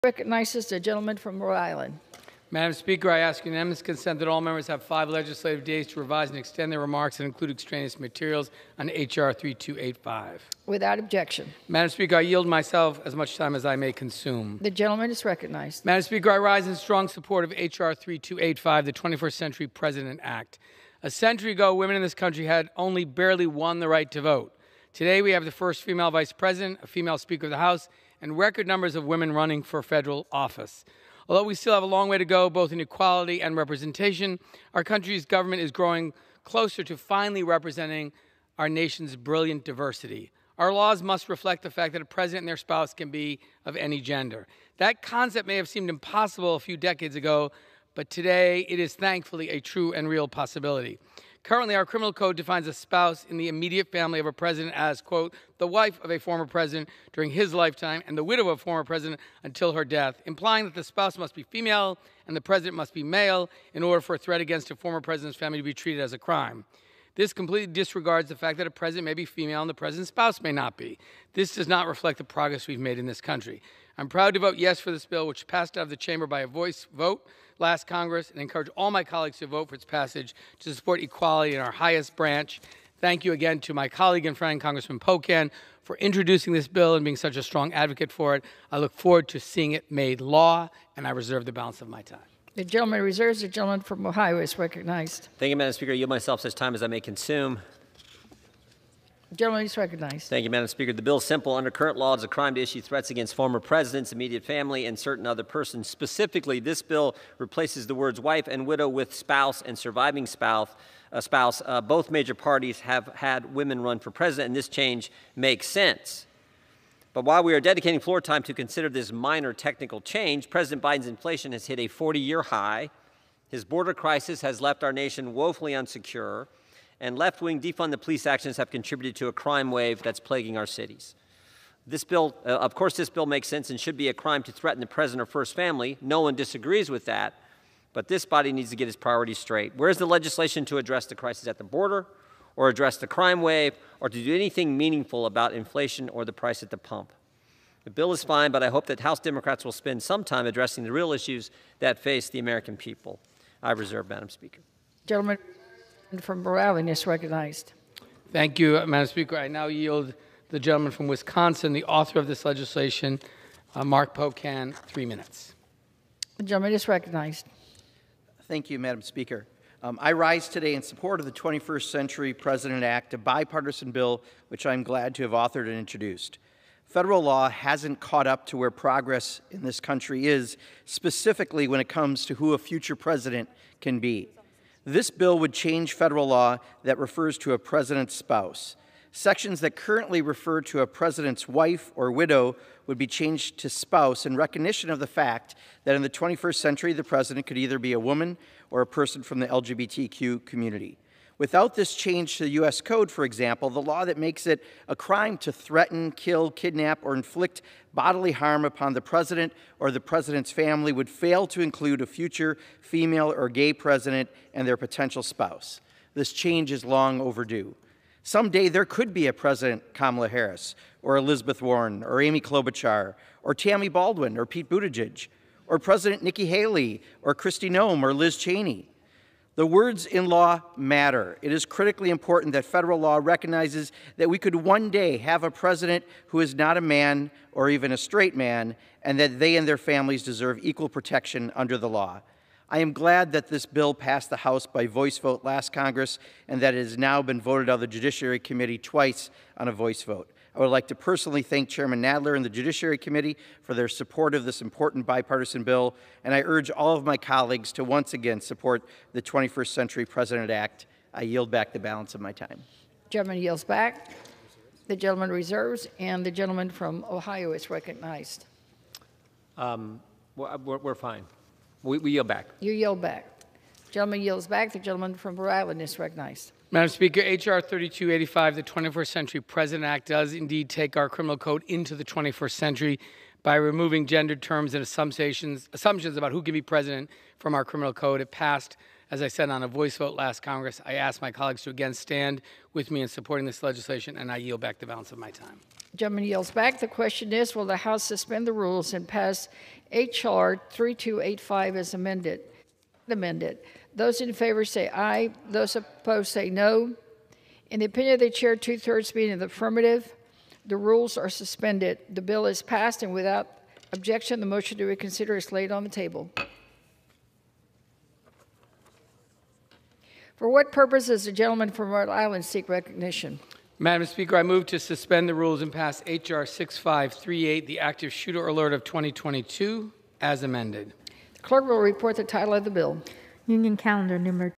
'This Body Needs To Get Its Priorities Straight': Jim Jordan Rips Democrats' Priorites
Though he agreed with the 21st Century President Act, Rep. Jim Jordan (R-OH) called for action he considered more important.